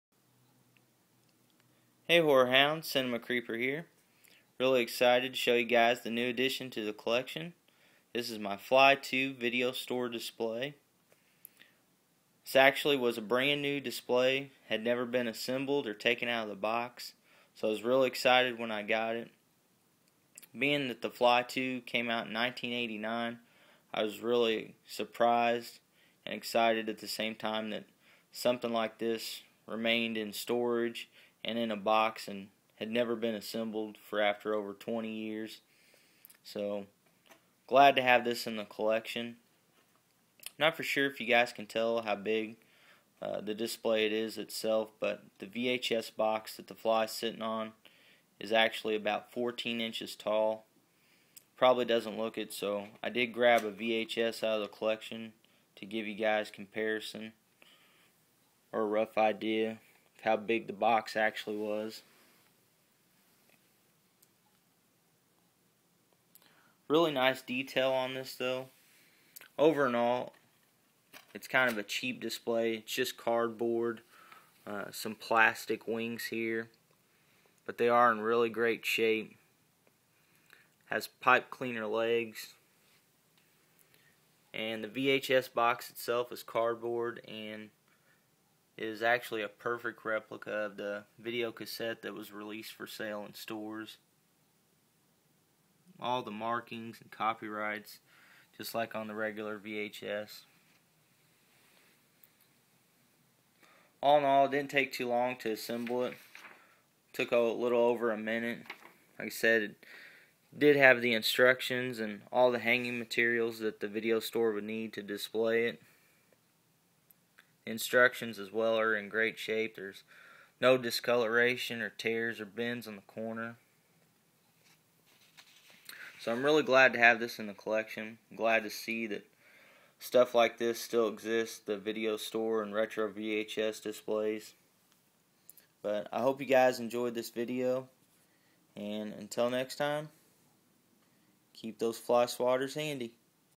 Rare Video Store Display: The Fly II
the fly 2 vhs video rental store display. You horror hounds and VHS collectors will enjoy seeing this!